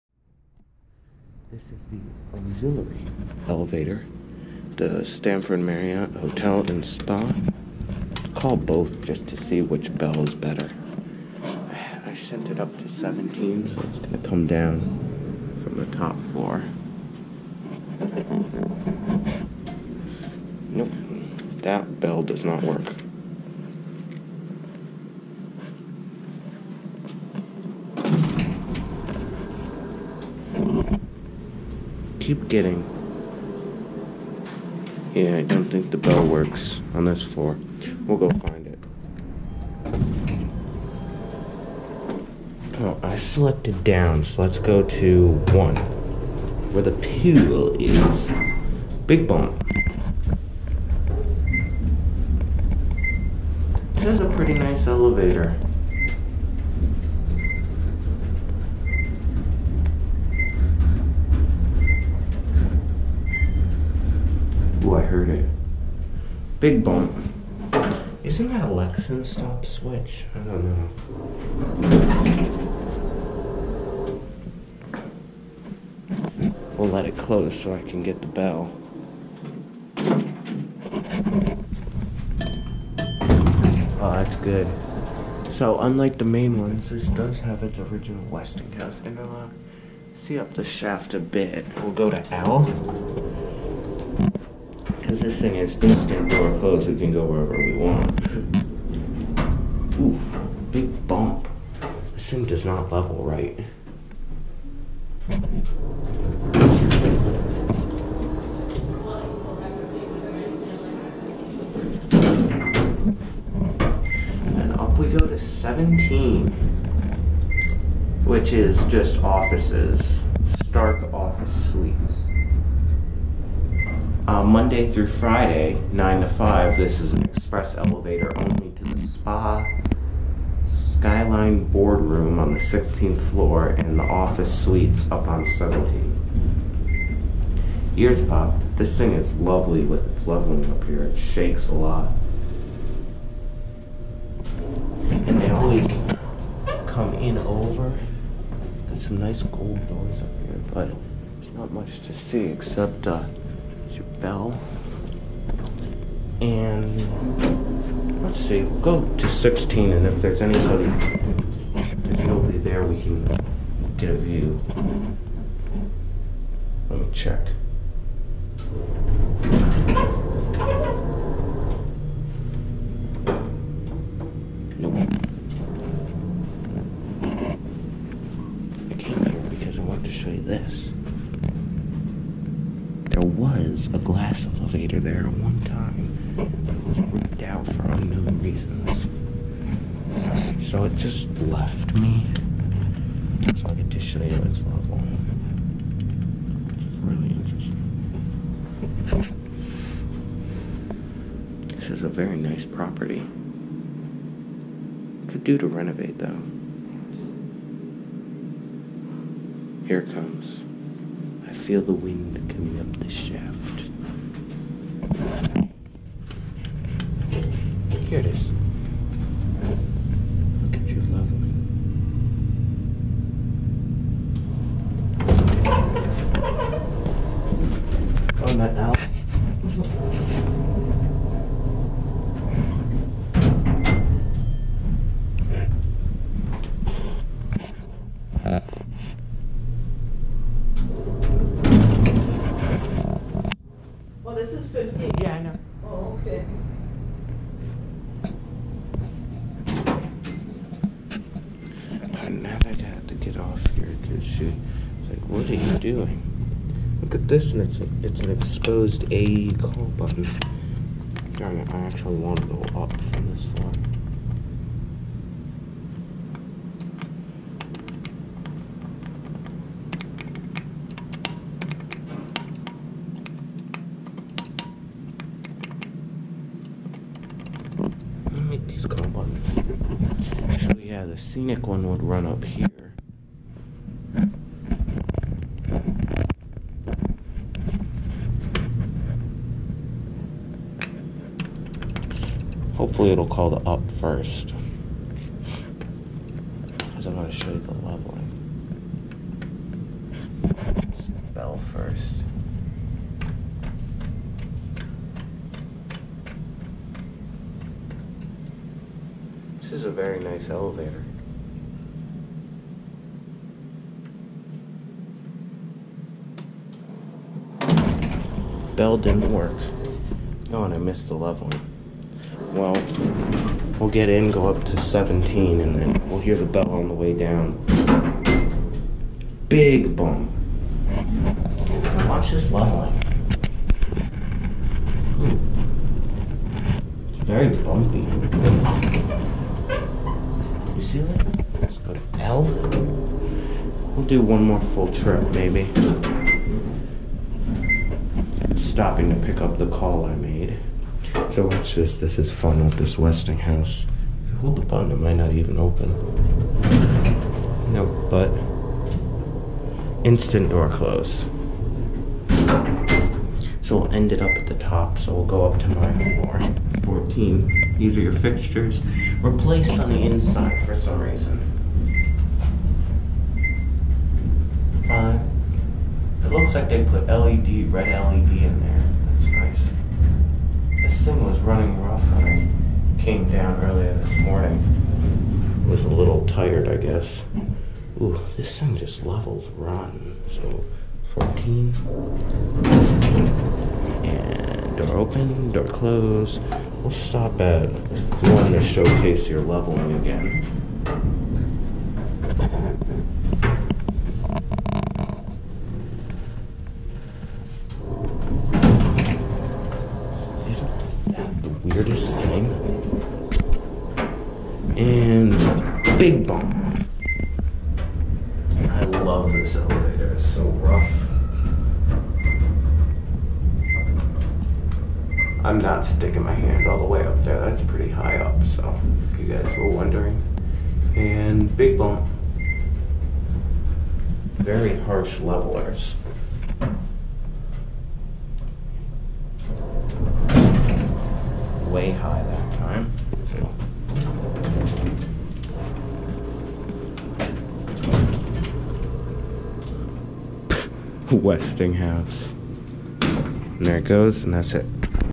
ORIGINAL Westinghouse Aux. Elevator @ The Stamford Marriott
This does have replaced button on the inside, but still has the original controller, motor, and bells!! Could be a final ride because the hotel is being renovated soon!!

Actually to be truthful this could use a modernization. Going up it always over-levels about 3in and comes back down!!